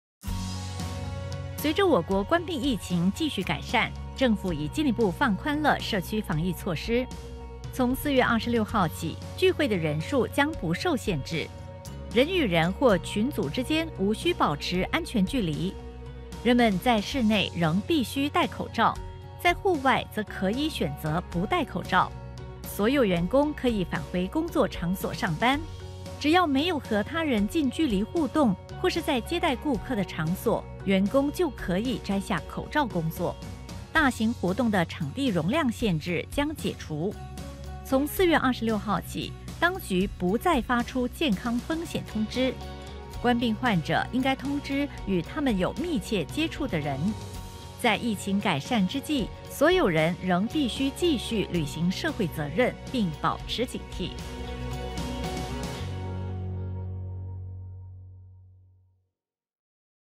Easing community measures (Chinese)
As Singapore’s COVID-19 situation continues to improve, the Government is further easing community measures. From 26 April, there will be no group size limits. Safe distancing is not required. Mask-wearing will continue to be required indoors and optional outdoors.